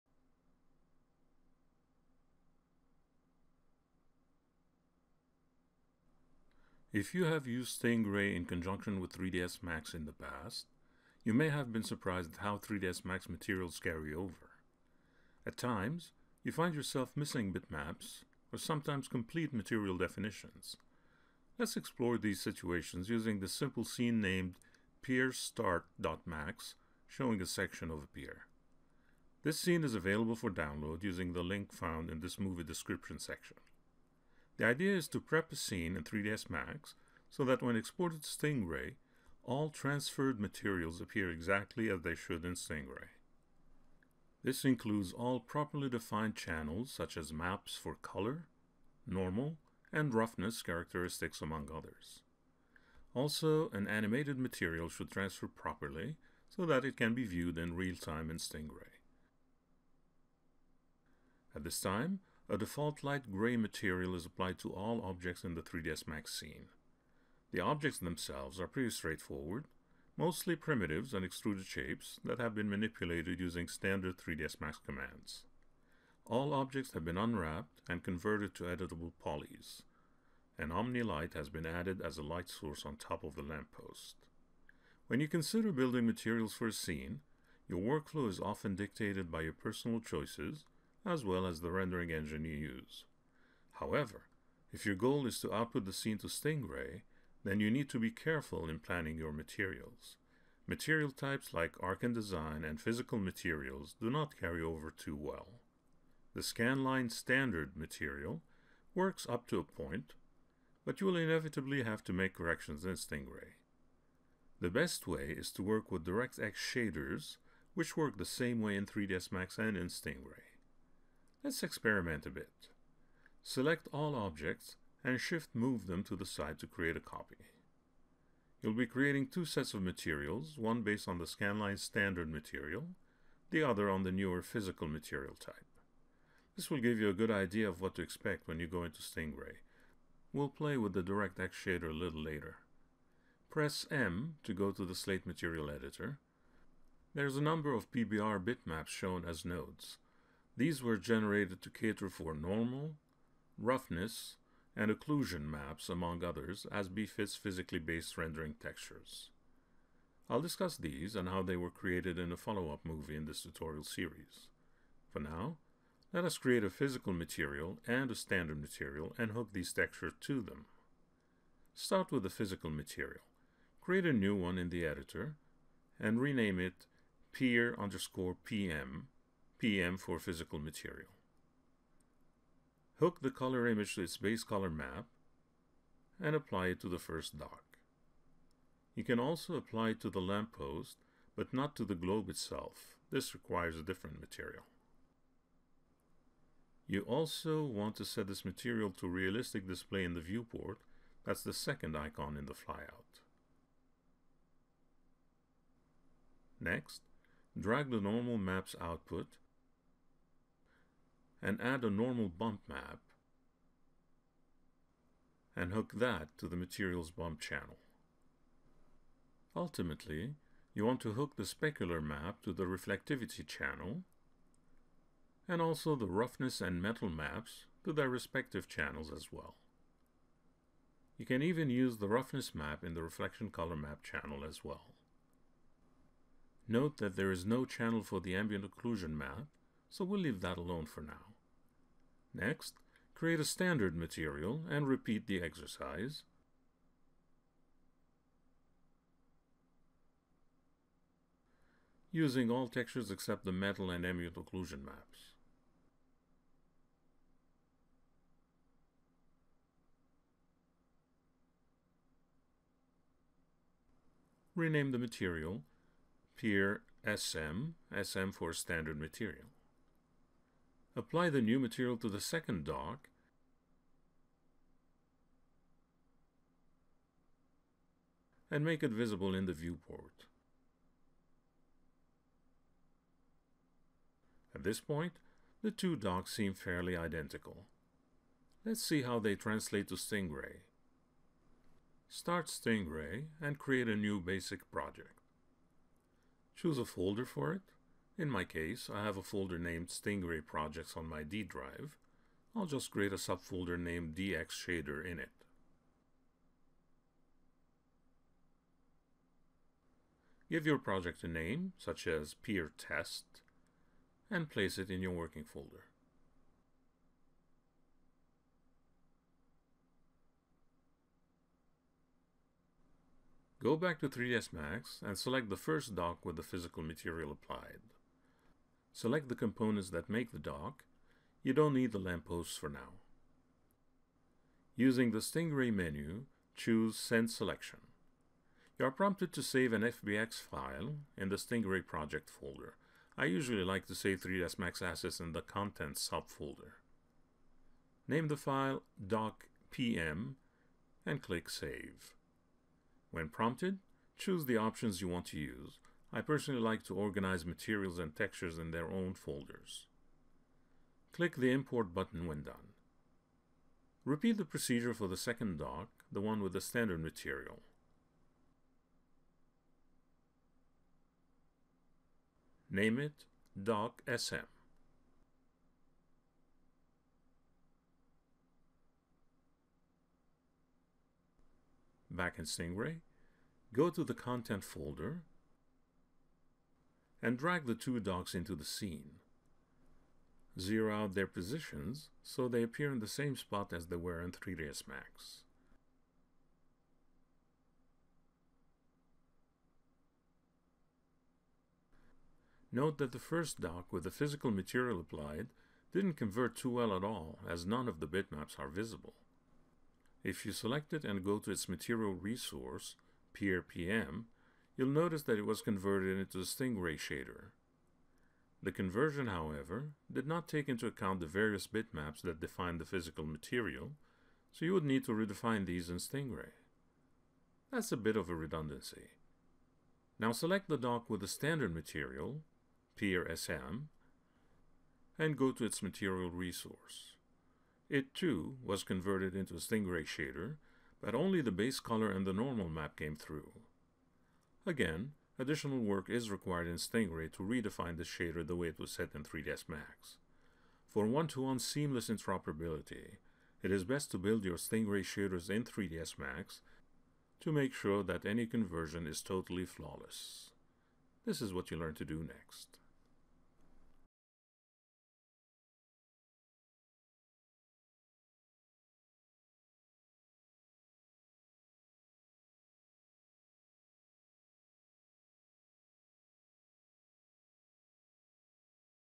3ds Max Shaders for Stingray - Part 1 - Introduction
This is a 3ds Max multi-part series where you will learn how to export materials from 3ds Max to Stingray. In this series, you will learn about the importance of 3ds Max’s DirectX Shader when exporting your 3ds Max materials to Stingray. This material type ensures that your 3ds Max materials translate exactly as designed into Stingray. In this Part 1 movie, you take a look at the scene you will be using and learn about standard material conversions.

Level: Intermediate
Recorded in: 3ds Max 2017.1 & Stingray 1.6

 This tutorial is intended for use with 3ds Max version 2017.1 Update or higher.